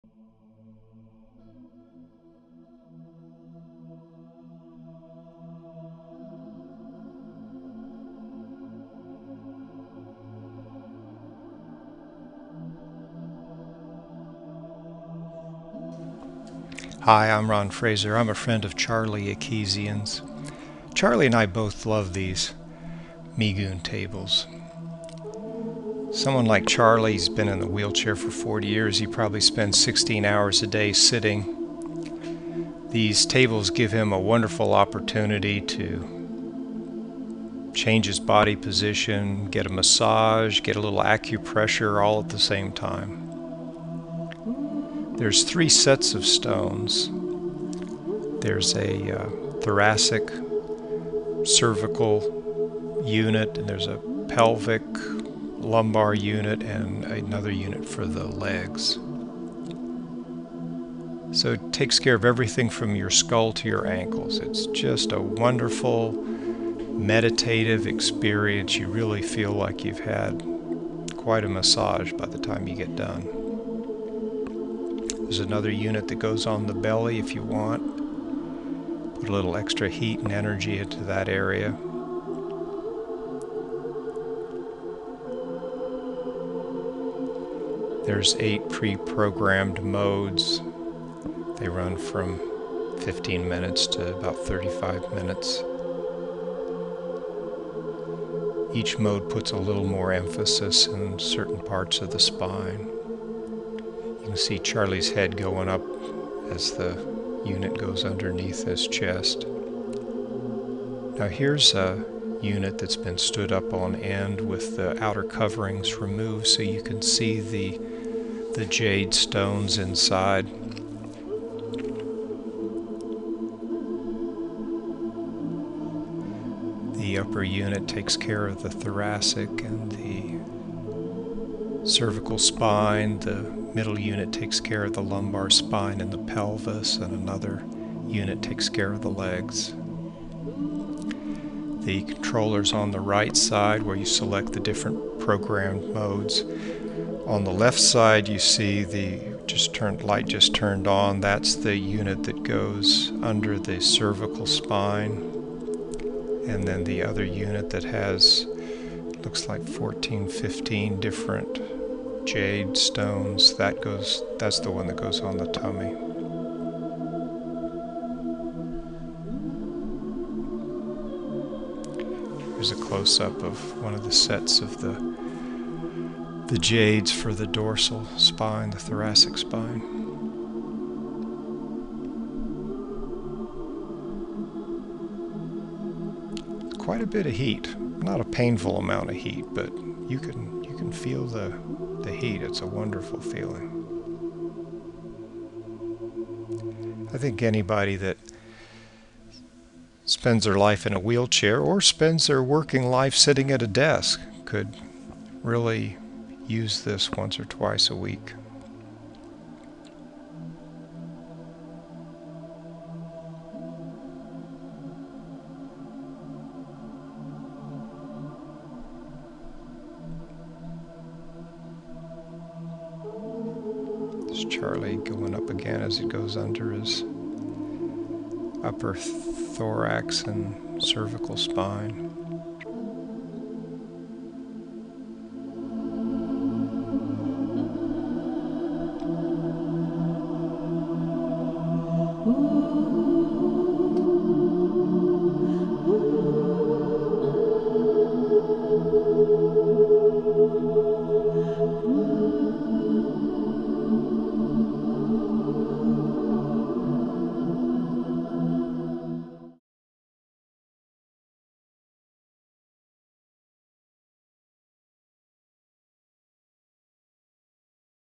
Charlie Ekizian on Migun Tables
Quadriplegic Charlie Ekizian, president of Wheelchair Sports and Recreation Association, loves Migun tables; the combination of massage, heat, and acupressure feels great after a few days in a wheelchair.

Charlie Ekizian has had an amazing life.  In 1968 he broke his neck in a diving accident and was told he'd be in a nursing home for the rest of his life.  He used athletics to regain much of his abilities.  He married, finished college, had a successful career and has spent the last forty years inspiring other mobility-impaired people to reach higher goals.  Charlie's website is